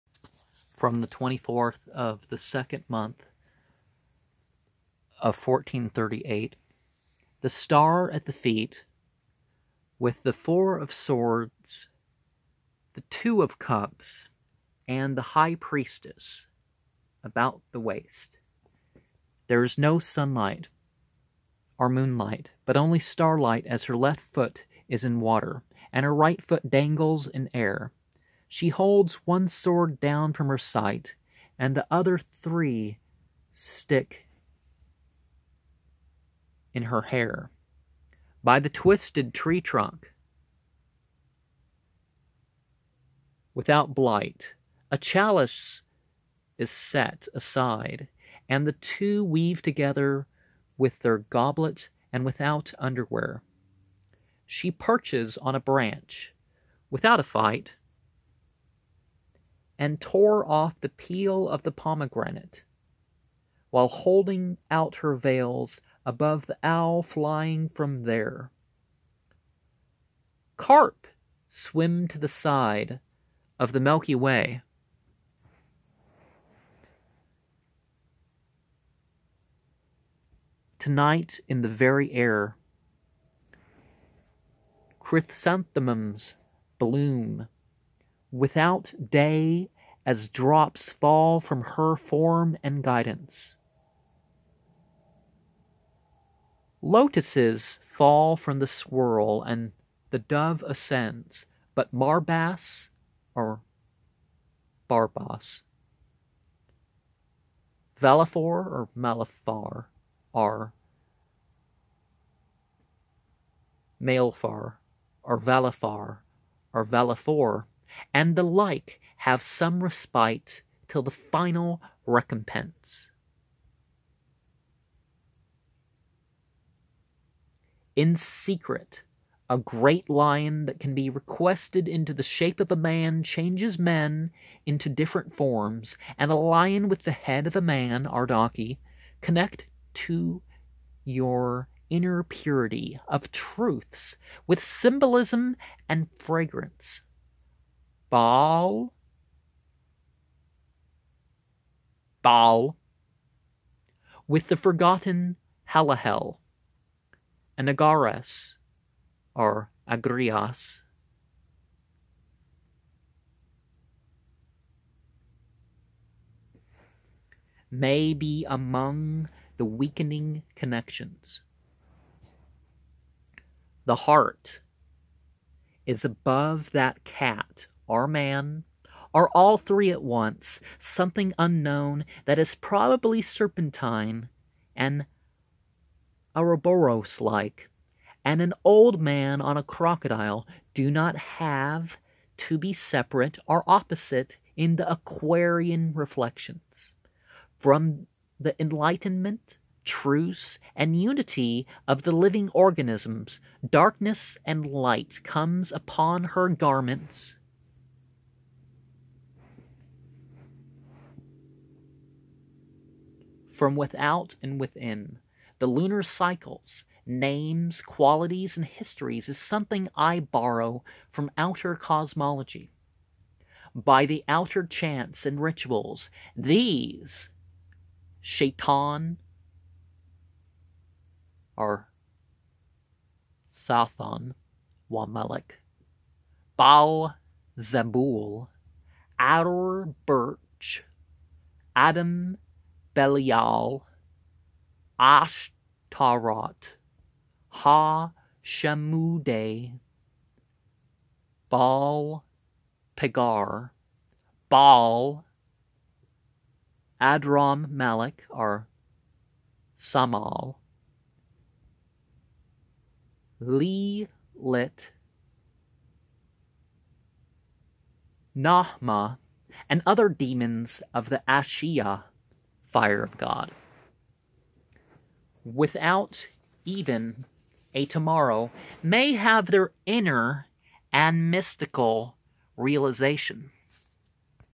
24/2/1438 Demonology That Manifested with The Shadowscapes Tarot Stephanie Pui-Mun Law Barbara Moore
The Star at the Feet with The Four of Swords,  the Two of Cups, and The High Priestess About The Waist was 24/2/1438 Demonology That Manifested with The Shadowscapes Tarot by Stephanie Pui-Mun Law and Barbara Moore.